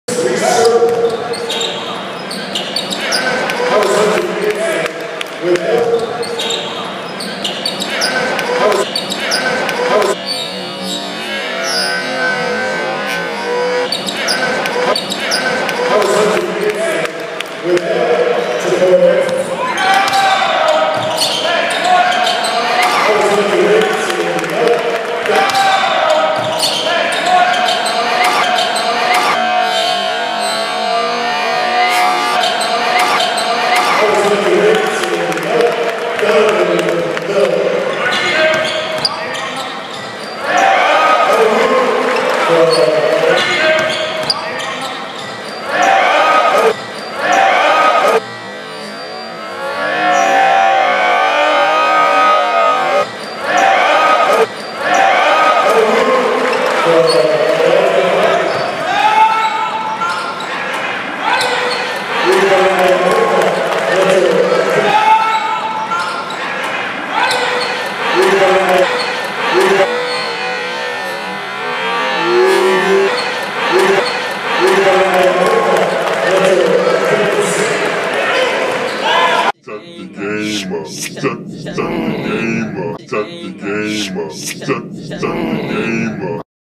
DeMatha's Hunter Dickinson's 3 POWERFUL 2-hand dunks & oop vs. Ireton.
DeMatha Catholic High School's Hunter Dickinson slides down the lane from the top of the key, catches a pass and throws down a powerful 2-hand dunk. Dickinson works his way free behind a zone defense, receives a pass and finishes with a powerful 2-hand dunk. Dickinson sneaks in behind the zone defense, as the ballhandler penetrates, and slams in a 2-hand alley-oop. Dickinson crashes the boards, battles for the rebound, snatches the ball and converts the possession with a 2-hand dunk.